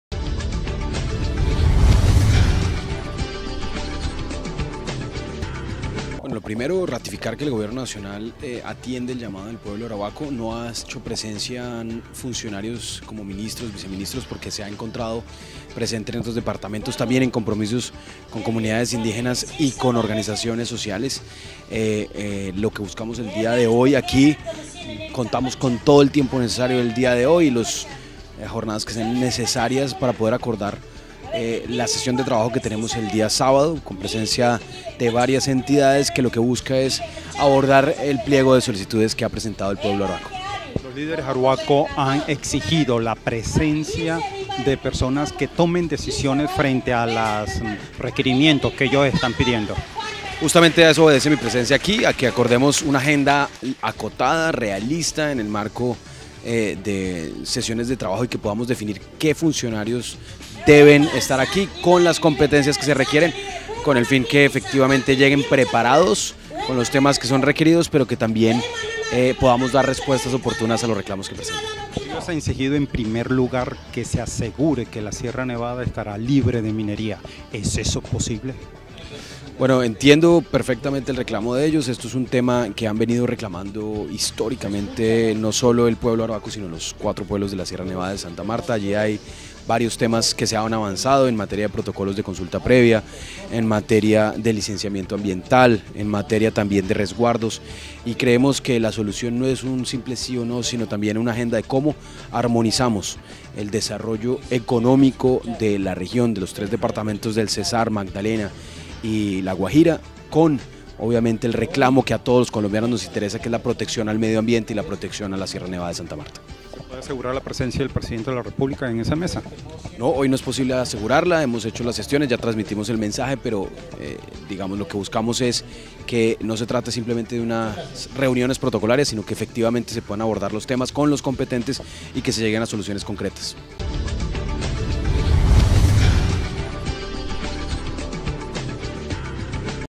Viceministro del Interior propone nueva reunión de alto nivel con Arhuacos - Luis Ernesto Gómez
Luis Ernesto Gómez Viceministro del interior en diálogo con el Pueblo Arhuaco para encontrar fórmulas de protección del territorio y de la cultura ancestral de la Sierra Nevada de Santa Marta.
"El reto de este diálogo es armonizar la conservación ambiental y cultural de la SierraNevada con el desarrollo de Cesar, Magdalena y Guajira" afirmó el Viceministro en su intervención.